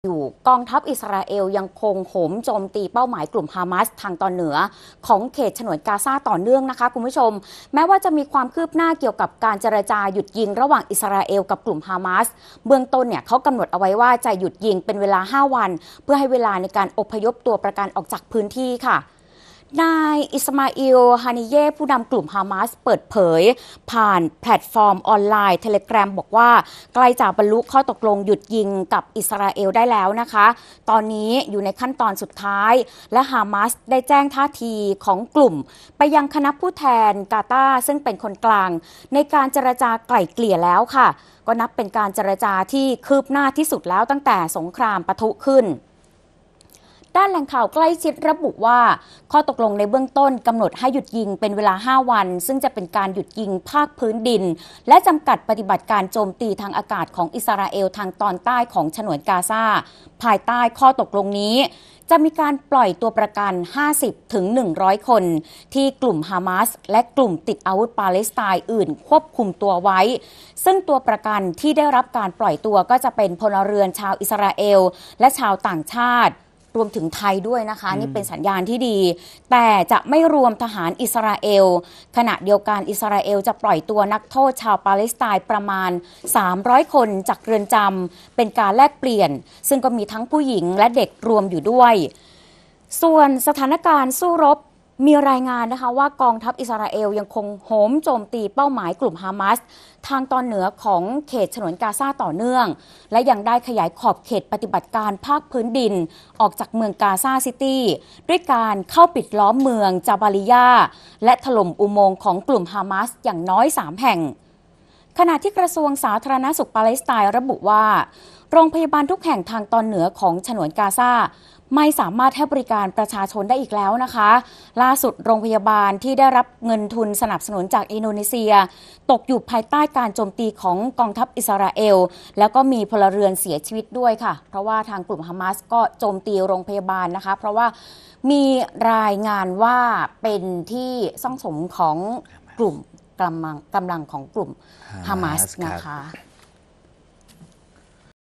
ผู้นำฮามาสเผยใกล้บรรลุข้อตกลงหยุดยิงกับอิสราเอล ข่าวเช้า วันที่ 22 พฤศจิกายน 2566
สถานการณ์ในตะวันออกกลาง กองทัพอิสราเอลยังคงโหมโจมตีเป้าหมายกลุ่มฮามาสทางตอนเหนือของเขตฉนวนกาซาต่อเนื่อง แม้มีความคืบหน้าเกี่ยวกับการเจรจาหยุดยิงระหว่างอิสราเอลกับกลุ่มฮามาส เบื้องต้นกำหนดให้หยุดยิงเป็นเวลา 5 วัน เพื่อให้เวลาอพยพตัวประกันออกจากพื้นที่